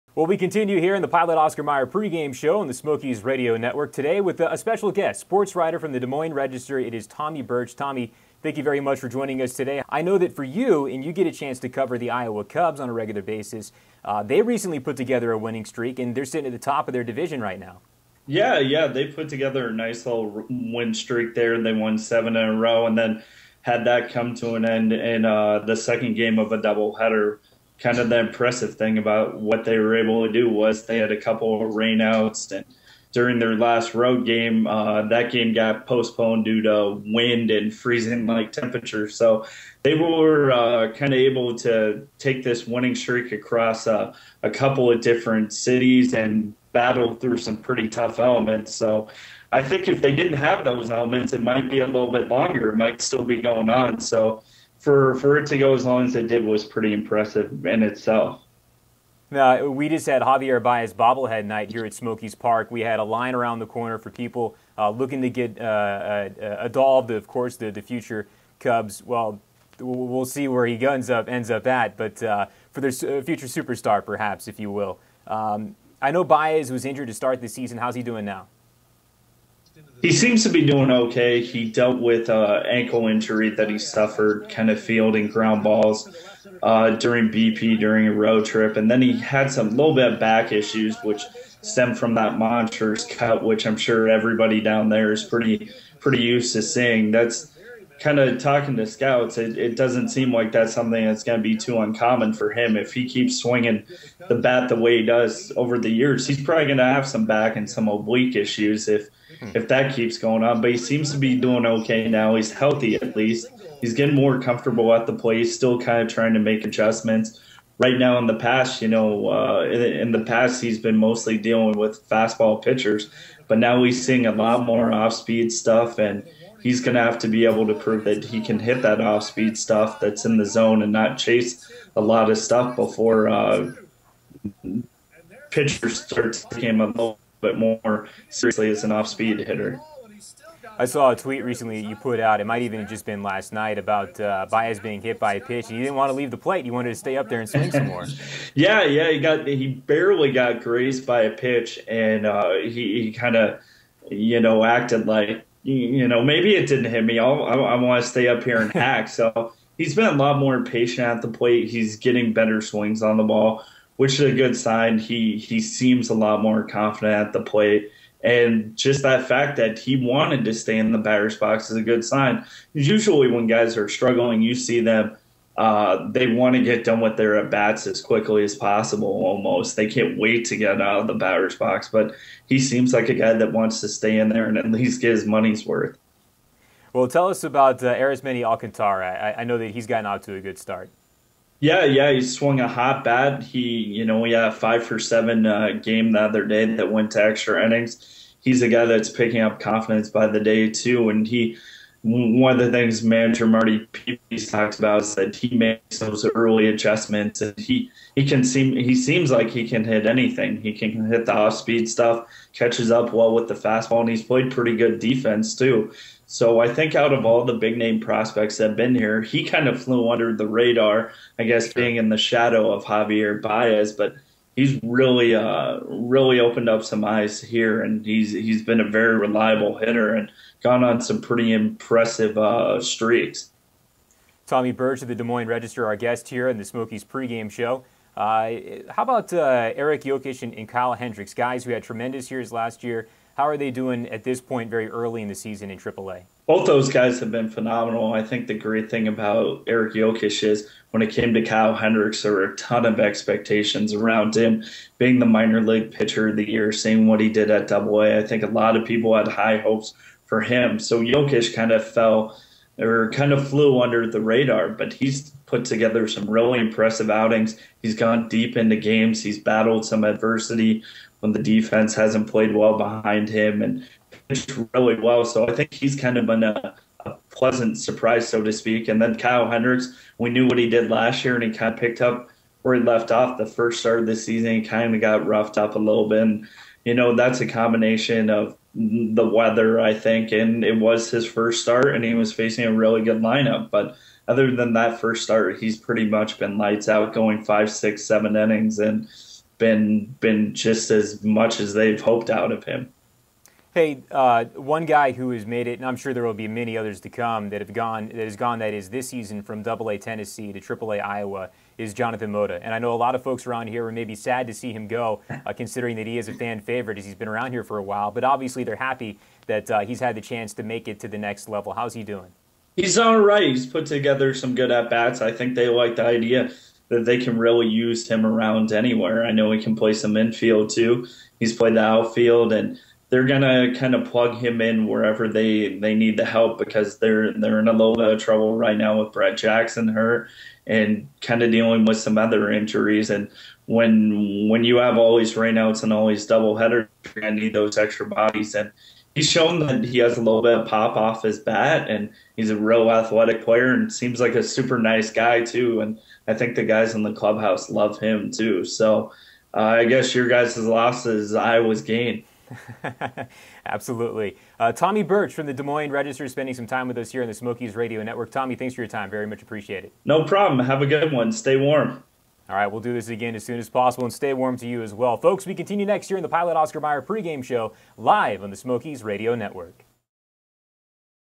Smokies Radio Network Interview with Tommy Birch of The Des Moines Register
Des Moines Register Sports Writer Tommy Birch spent some time with the Smokies Radio Network to discuss former Smokies Javier Baez, Arismendy Alcantara, Kyle Hendricks, Eric Jokish and Jonathan Mota and their respective seasons with the Triple-A Iowa Cubs.